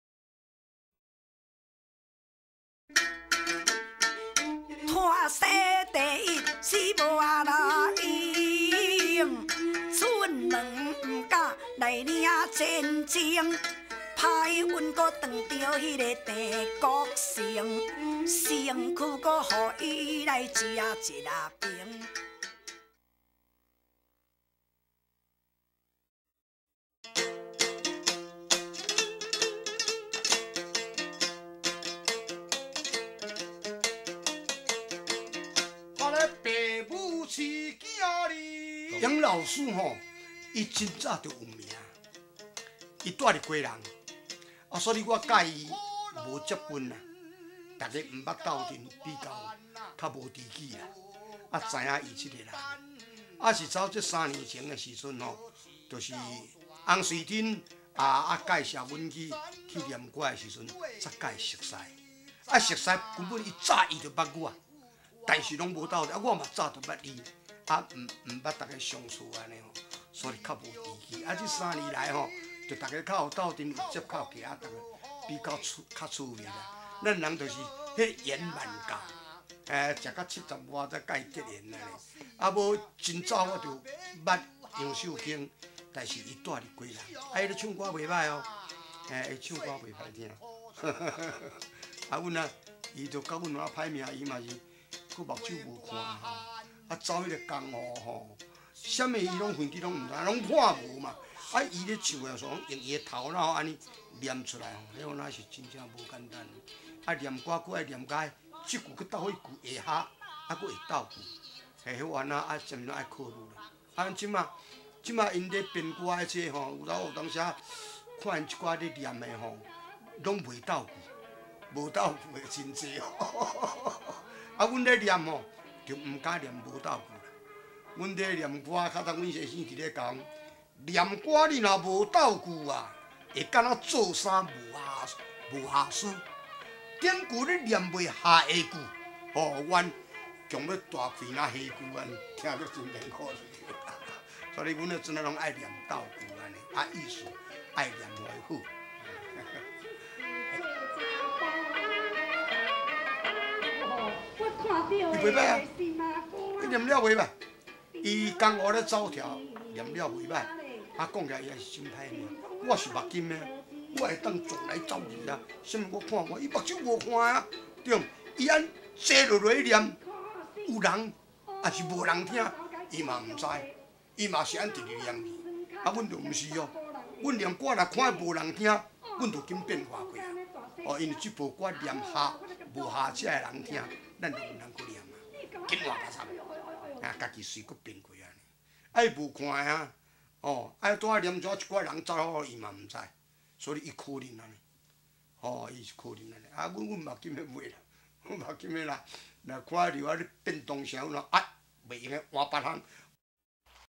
台灣唸歌團 同行惜情 王玉川.鄭來好.陳美珠.王郭招治2004.12.29
同行人士王玉川, 鄭來好, 陳美珠.王郭招治 接受訪問佮表演唸唱技藝的風彩